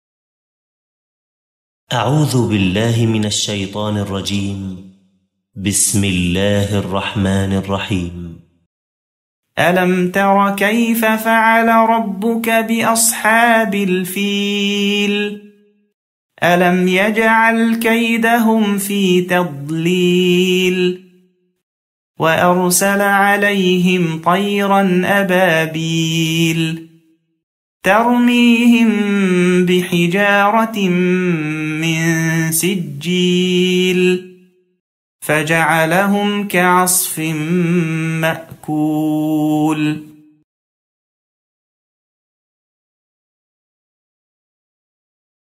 105 ALFil 〔The Elephant〕 سورة الفيل English Muhsin Khan and TaqiUdDin AlHilali Radwan
105-ALFil 〔The Elephant〕-سورة الفيل-English-Muhsin Khan and TaqiUdDin AlHilali-Radwan... [Have you (O Muhammad (Peace be upon him)) not seen how your Lord dealt with the Owners of the Elephant? [The elephant army which came from Yemen under the command of Abrahah Al-Ashram intending to destroy the Ka'bah at Makkah].(1)Have you (O Muhammad (Peace be upon him)) not seen how your Lord dealt with the Owners of the Elephant? [The elephant army which came from Yemen under the command of Abrahah Al-Ashram intending to destroy the Ka'bah at Makkah].(1)Did He not make their plot go astray?(2)And sent against them birds, in flocks,(3)Striking them with stones of Sijjil.(4)And made them like an empty field of stalks (of which the corn has been eaten up by cattle).(5)]

Quran ... [Do they not then consider the Quran carefully? Had it been from other than Allah, they would surely have found therein much contradictions.]

Surah Name: 105-ALFil 〔The Elephant〕-سورة الفيل
Language: Arabic & English - English
Translator: Muhsin_Khan and TaqiUdDin_AlHilali